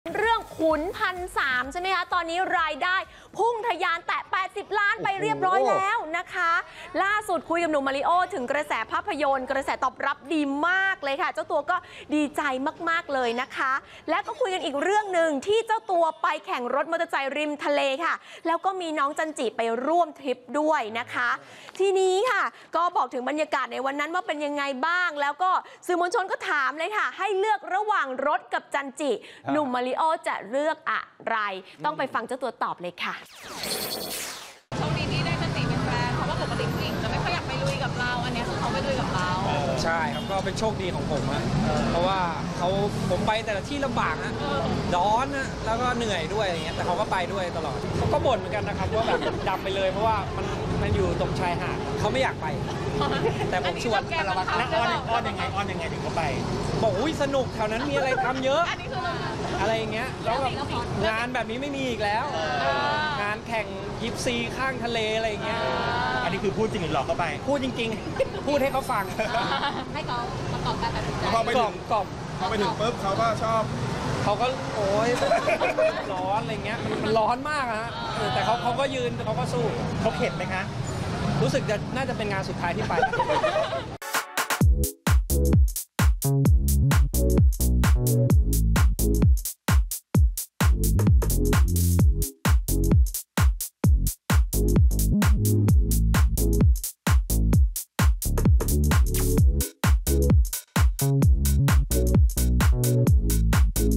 เปิดโหมดหวาน “มาริโอ้” รับ โชคดีได้ “จันจิ” เป็นแฟน | Apop Today
ท่ามกลางดราม่าลดรอบฉายภาพยนตร์ “ขุนพันธ์ 3” แต่บอกเลยว่ากระแสตอบรับกลับดีสุดๆ โกยรายได้ทะยานสู่ 80 ล้านบาทแล้ว งานนี้พระเอกหนุ่ม “มาริโอ้ เมาเร่อ” ถึงกับออกปากดีใจสุดๆ นอกจากนี้ยังเล่าถึงทริปแข่งรถมอเตอร์ไซค์ริมทะเล ที่หวานใจสาว “จันจิ จันจิรา” ไปร่วมทริปคอยให้กำลังใจด้วย